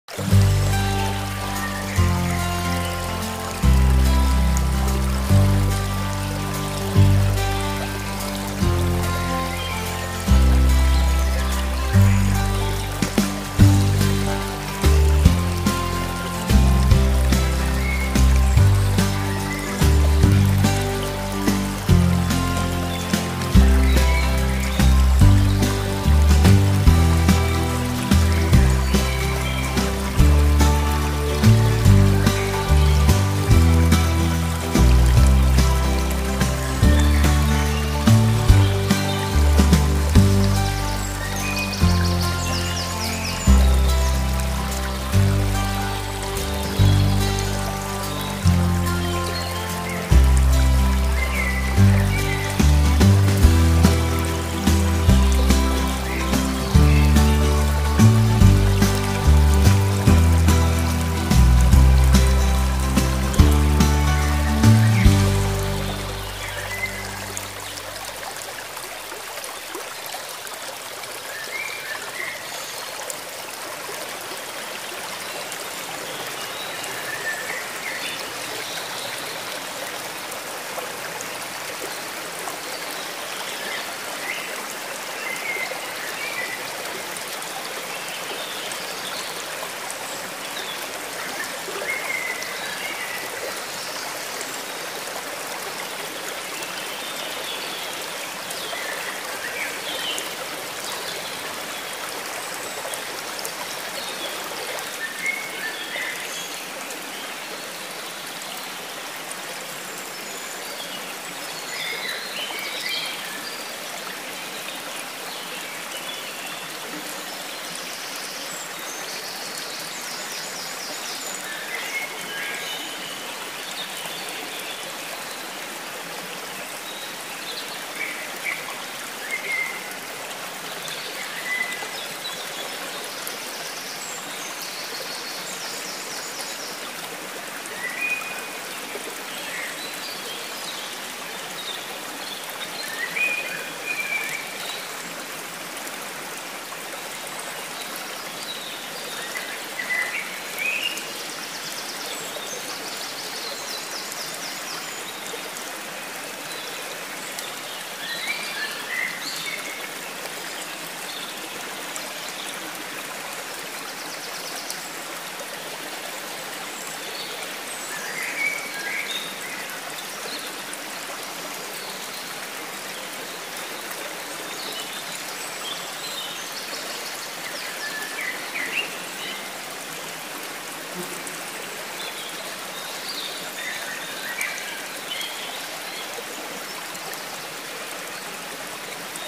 Rivière du printemps
Vidéo statique montrant le courant avec une petite plage de gravier et de pierres de riviere a mes pieds et aussi de l'autre coté en face.gros soleil.je me suis placé juste à côté du ruisseaux qui sort du bois avec des rochers ce qui fais beaucoup de bruit d'eau...mais le son n'étais pas bon alors je l'ai remplacé.🐬